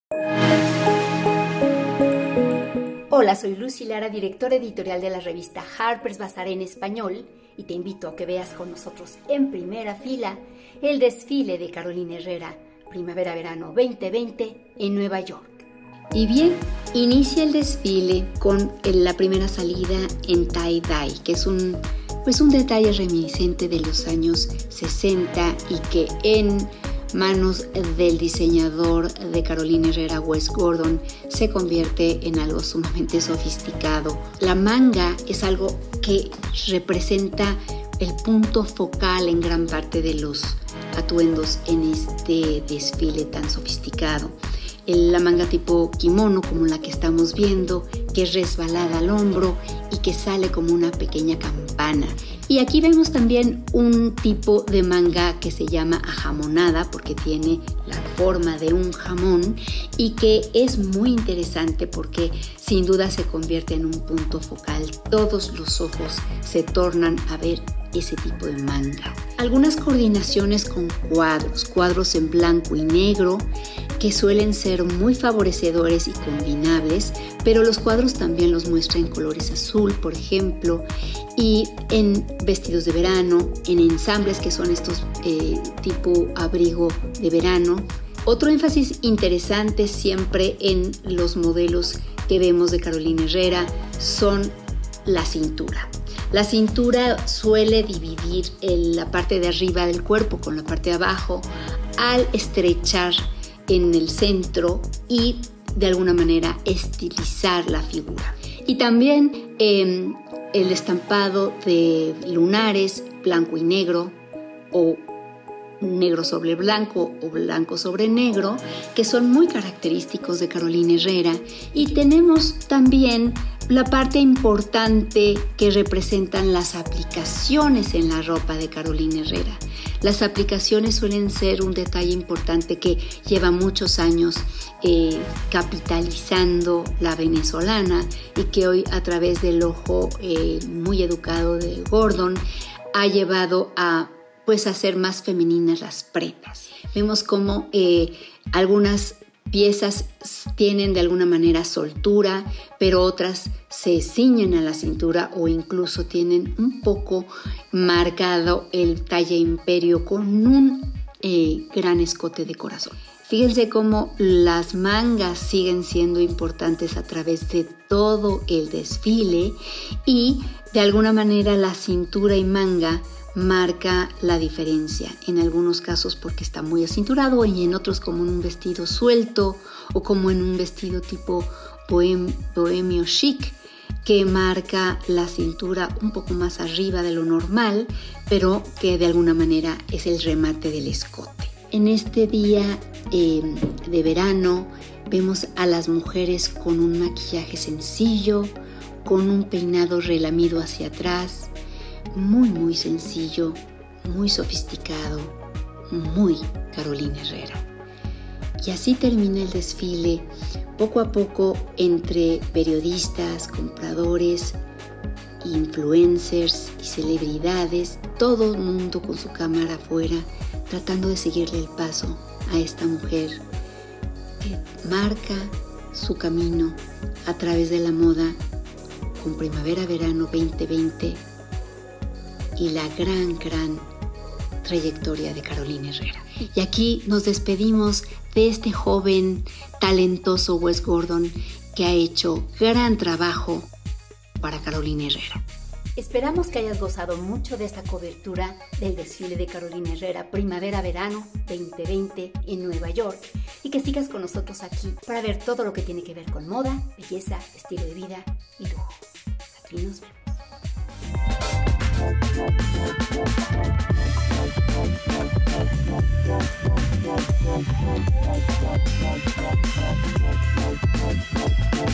Front Row: Lucy Lara reacciona al desfile de Carolina Herrera P/V20
La editora de Harper’s Bazaar en Español nos lleva al front row del desfile de Carolina Herrera PV20.